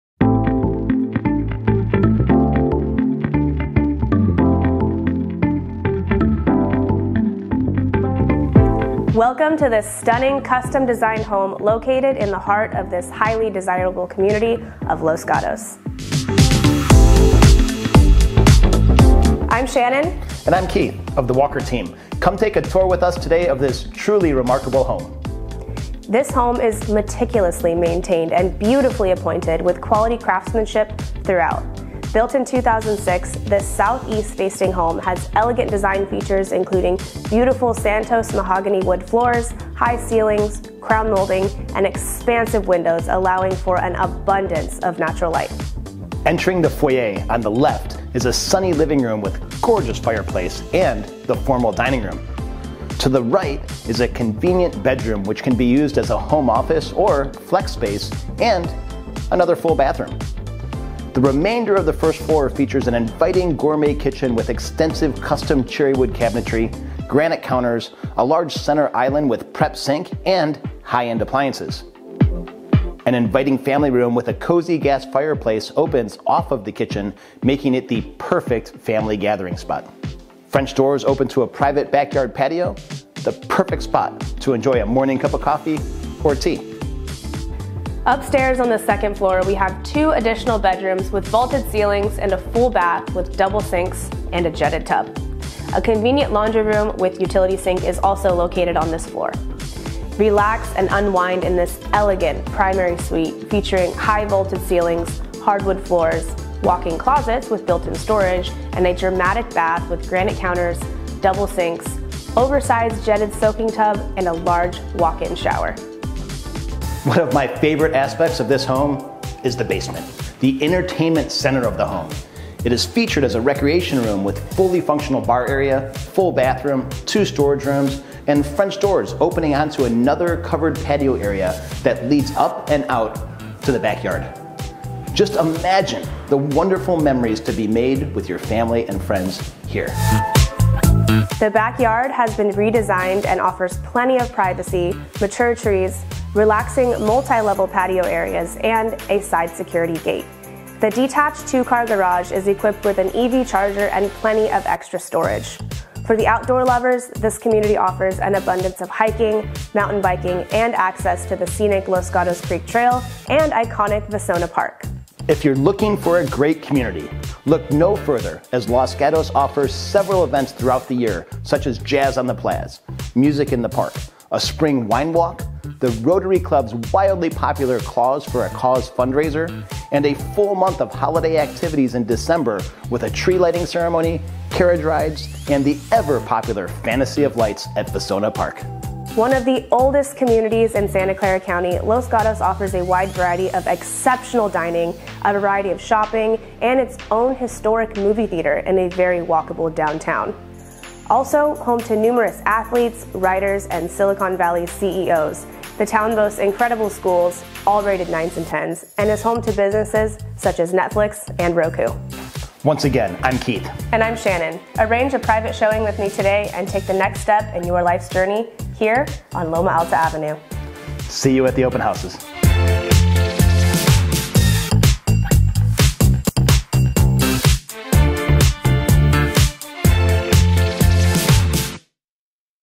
177 Loma Alta Ave Los Gatos, CA 95030
I present to you 177 Loma Alta Ave Los Gatos, CA 95030. A gorgeously updated single-family home in the highly desirable Los Gatos, CA.

4 bed 4 bath
3000+ sq ft
Improved and finished basement
$3,995,000